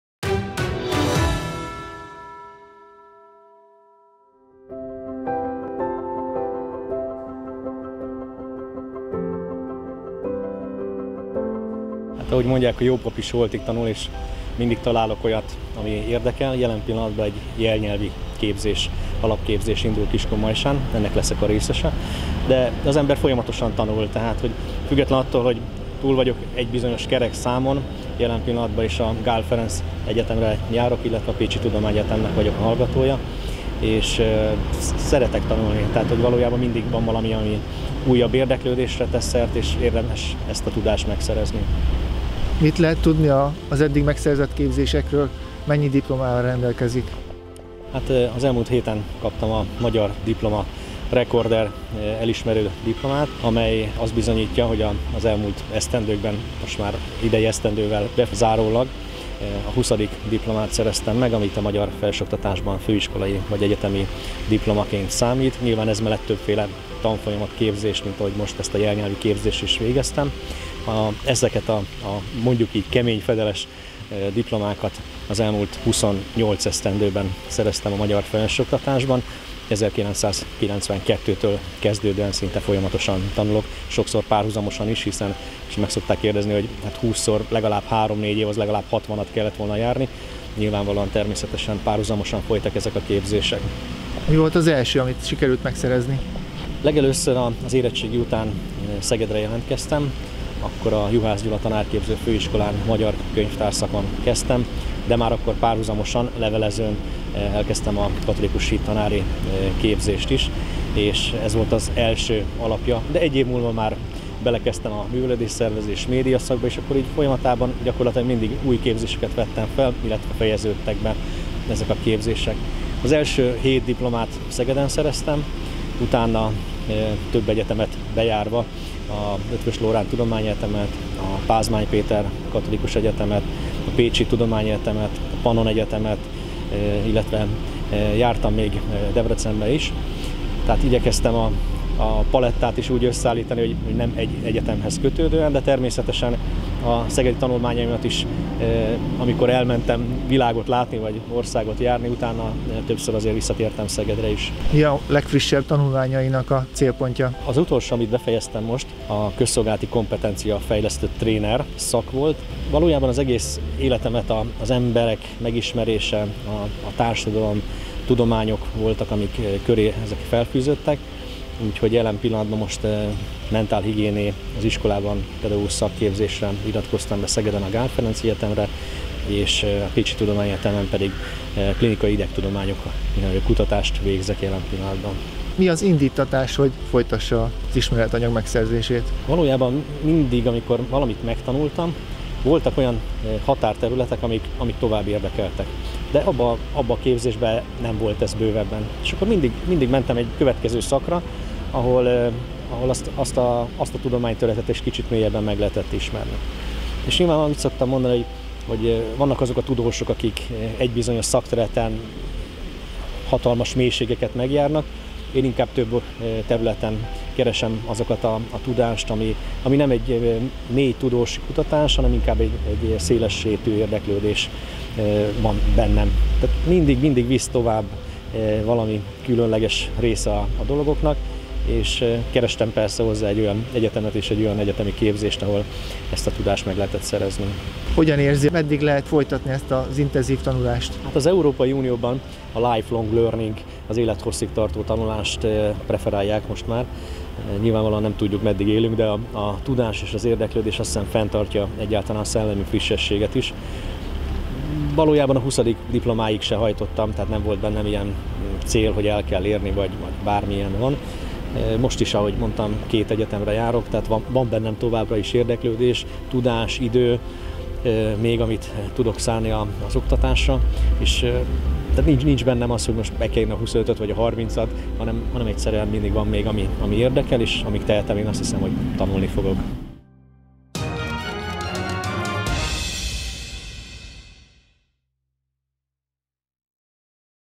MAGYAR REKORDER DIPLOMÁT VEHETETT ÁT KAPUS KRISZTIÁN
Kapus Krisztián, a majsai kistérség korábbi országgyűlési képviselője olyan, mint a jó pap; azaz holtig tanul. Kiskunfélegyháza volt polgármestere jelenleg egy jelnyelvi tanfolyamra jár Kiskunmajsán, de mindezzel párhuzamosan egyetemi tanulmányokat folytat és a doktori dissztertációjára is készül. Kapus Krisztián a diplomáinak számát tekintve hivatalosan is magyar rekorder, nemrégiben vehette át huszadik oklevelét, amit a magyar felsőoktatásban szerzett. A KVT Médianak adott interjújában Kapus Krisztián beszélt arról is, hogy mi motiválja a tudásának folyamatos gyarapítására.